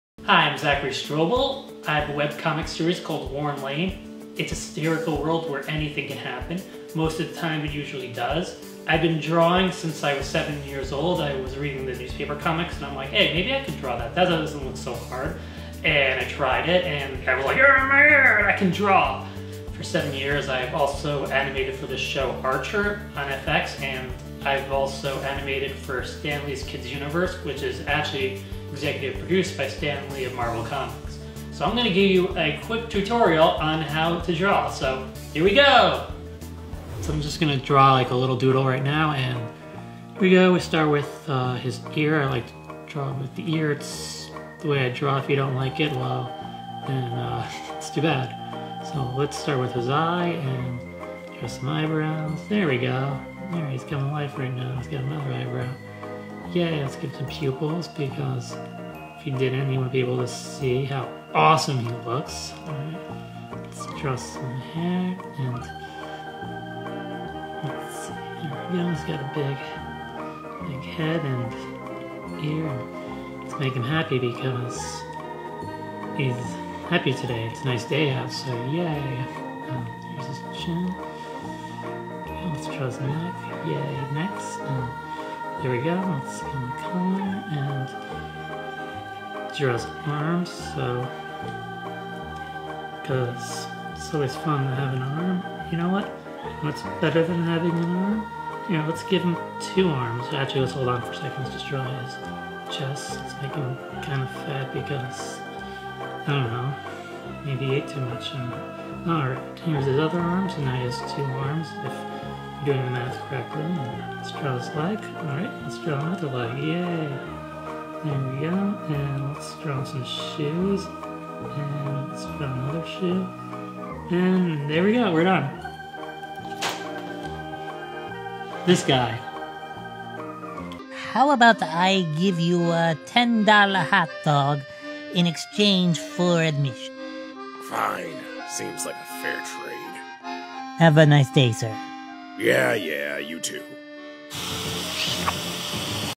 "Warren Lane" Book by Zachary Strobel w/ Eddie Deezen Now Available On Amazon & Kindle (Link Below)
I wrote a comic book full of unused ooozetoon ideas. The book is like "The Far Side" meets "MAD Magazine" with its 75 satirical, gut-busting comics.
The foreword is written by Eddie Deezen, who worked for every Hollywood studio under the Sun but is notably known for voicing Mandark on "Dexter's Laboratory" and Eugene Felsnic in "Grease" (1978).
Thank you, Peter Darbey for filming and editing this awesome promo.